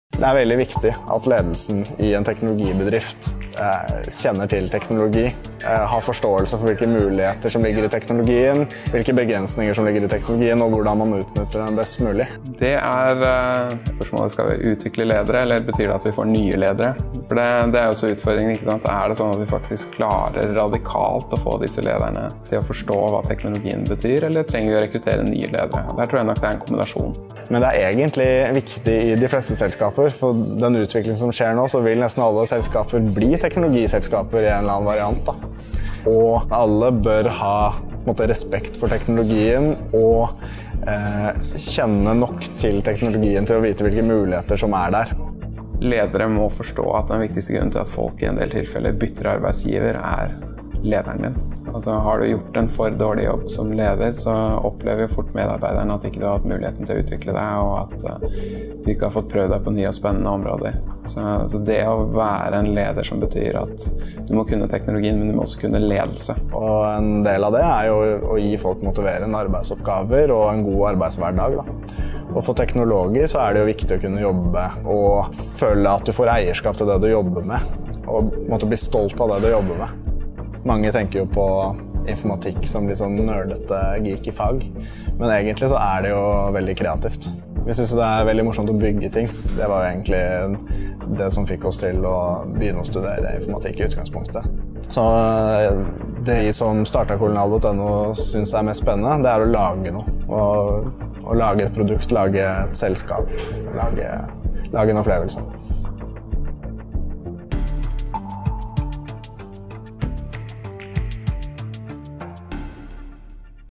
Informatikk: digital økonomi og ledelse (bachelor)
Når stadig flere bransjer digitaliseres, er det nødvendig at også de som leder bedrifter vet noe om teknologien. Det er økende etterspørsel etter den kombinasjonen av evner man utvikler her, og som kan brukes i høyteknologiske bedrifter som IKT- og programvareselskaper, konsulentselskaper og andre selskaper. Les mer om studieprogrammet

Produsert av: Red Ant Productions AS
Regissør: Anders Bølling
Produsent: Joachim Thingnes
Fotograf: Kristoffer Grindheim og Johan Peter Kraugerud
Klipper: Carl Christian Dalaaker